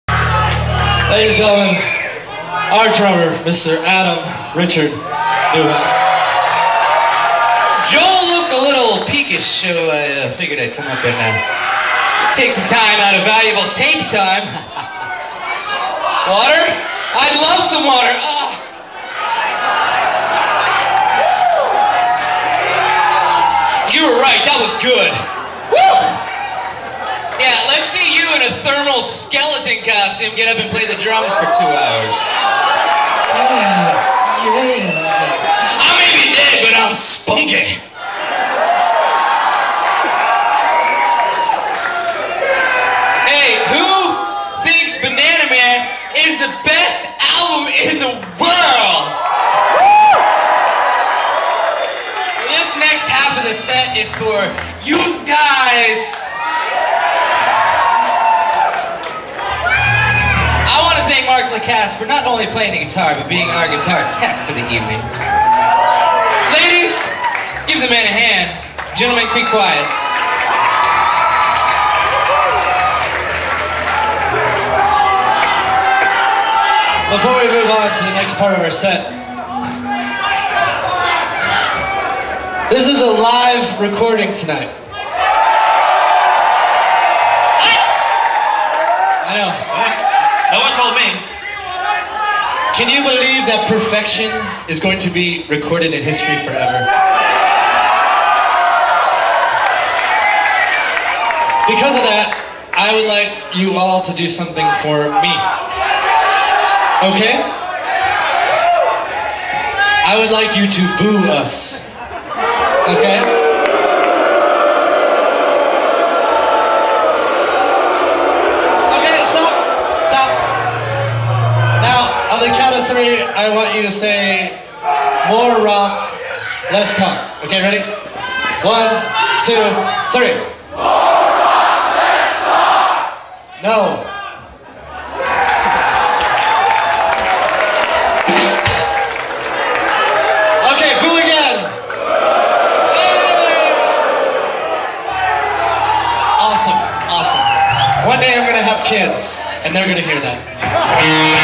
Ghoti Hook - "I want you to 'boo' us."
Ghoti Hook's final performance at Cornerstone 2002.